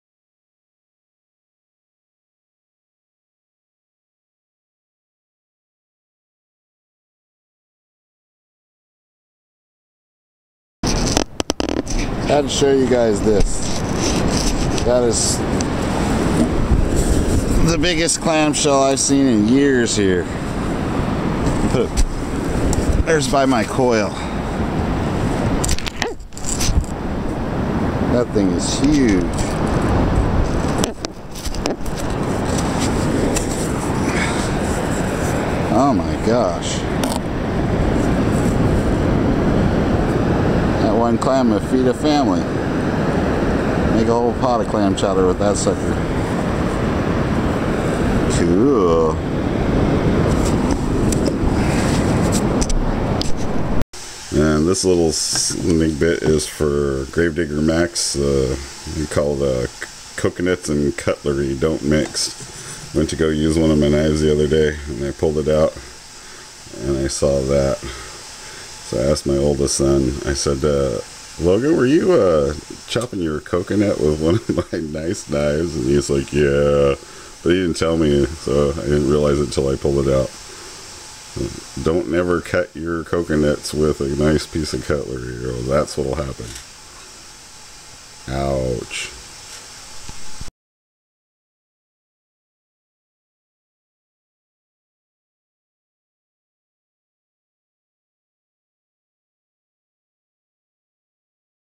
GIANT CLAM & Coconuts & Cutlery !
real sanded in went out by myself last night for a low tide spin to Pismo walked for miles not one signal not one! but ran across the biggest clam shell i think i have ever seen it really was the size of a large abalone  wow !2014 is off to kind of a bumpy start so far  detecting wise i'm sure things will pick back up soon :) take care guys i'll keep at it & try to get you guys that are snowed in some videos up ASAP :)